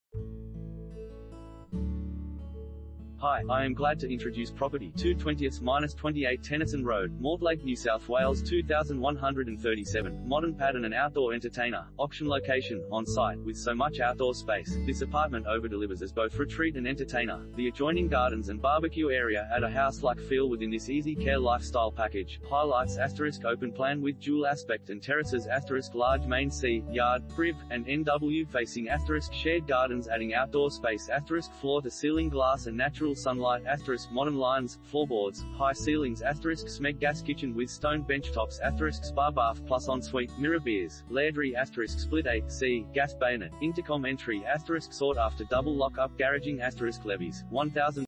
2/20-28 Tennyson Road, Mortlake NSW 2137 | AuPropertyReport.Com
2/20-28 Tennyson Road, Mortlake NSW 2137
Property Type: apartment
Bedrooms: 2
Bathrooms: 2
Car Space: 2
Modern pad &amp; an outdoor entertainer